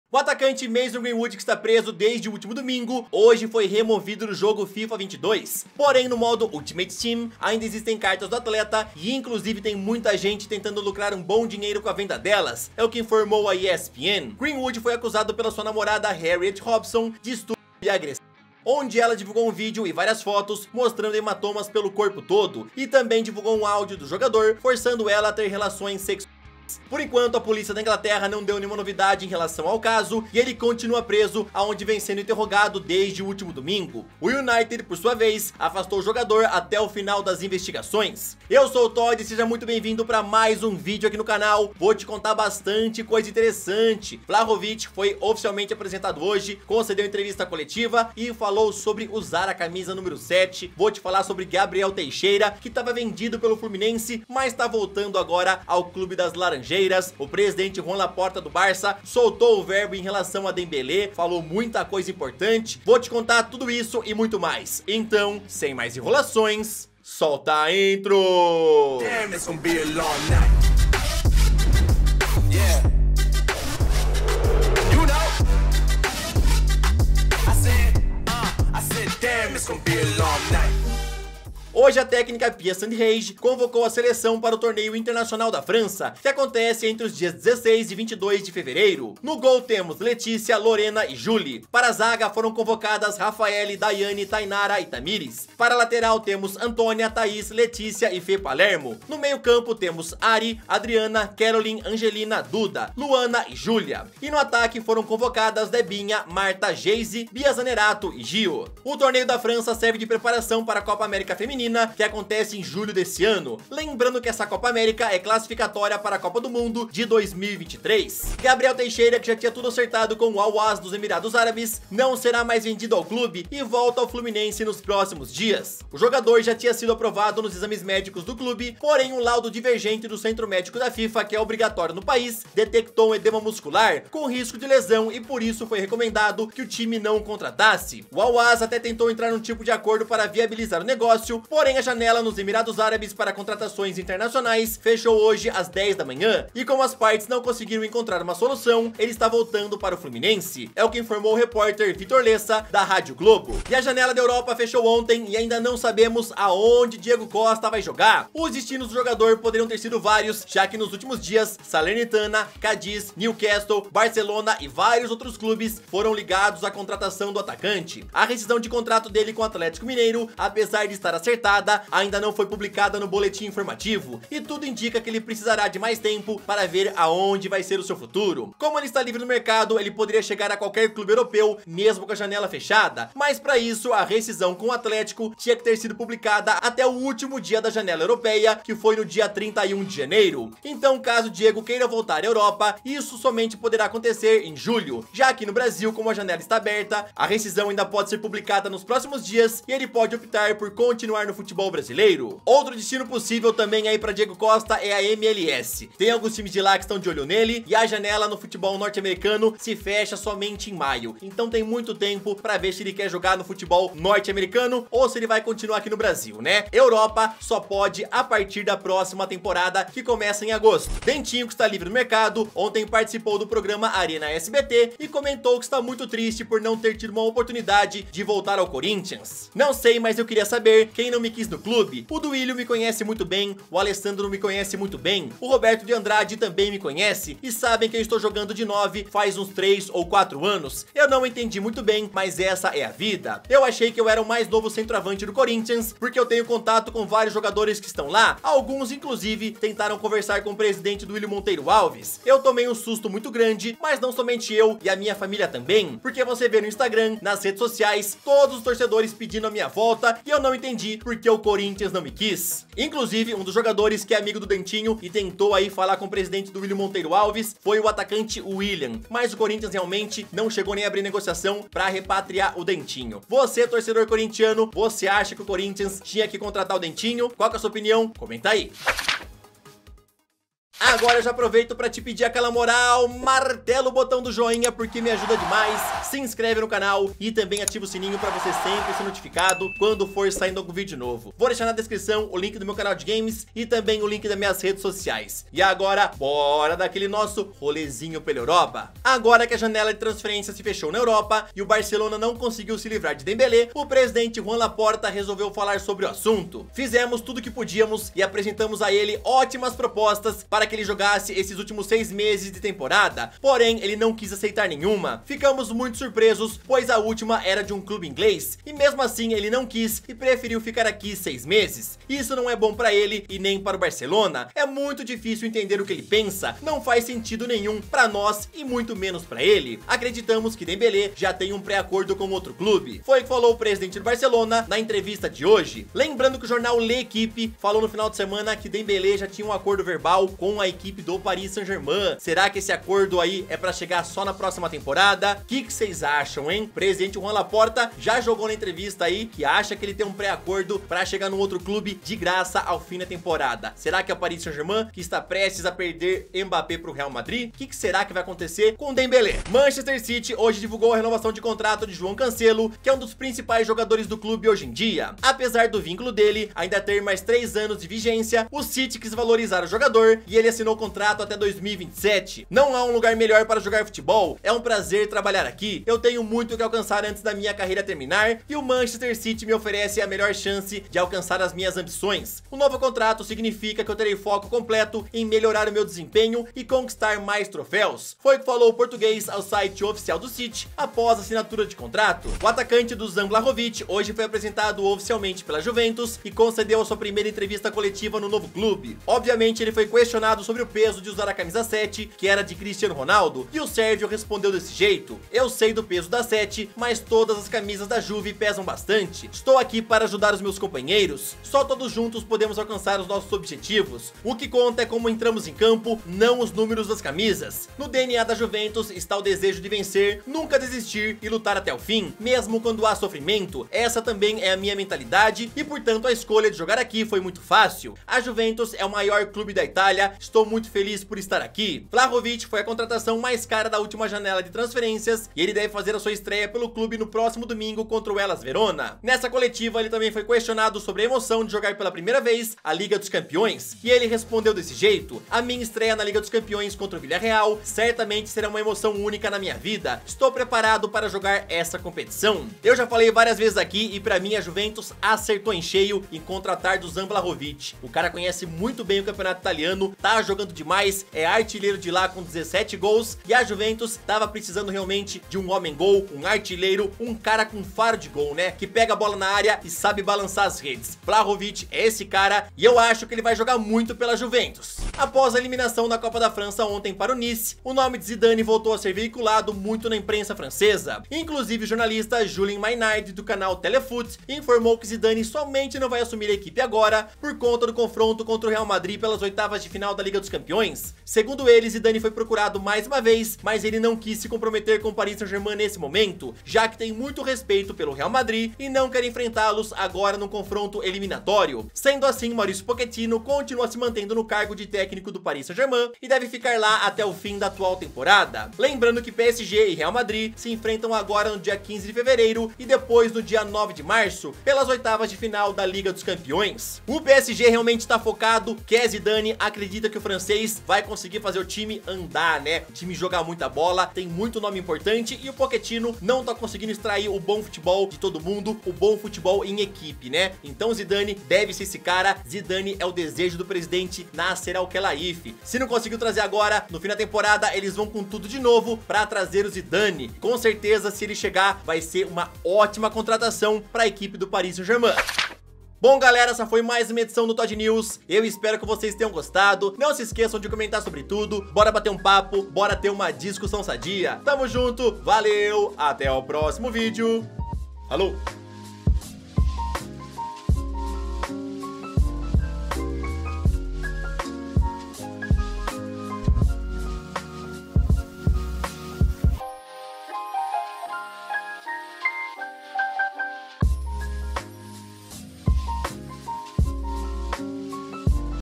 REVELADO o motivo do NÃO de ZIDANE l "DEMBÉLÉ fechou com outro clube" LAPORTA l DENTINHO fala tudo
Hoje tá lotado de notícia boa...
➡️ Mason Greenwood deletado do FIFA 22
➡️ Dentinho chateado com Corinthians
➡️ Gabriel Teixeira voltando ao Fluminense
➡️ revelado o motivo do não de Zidane
➡️ Laporta fala tudo sobre Dembele
➡️ Vlahovic "a 7 não vai pesar pra mim"
➡️ Diego Costa vai pra onde ?
Isso e muito mais no TODE NEWS !!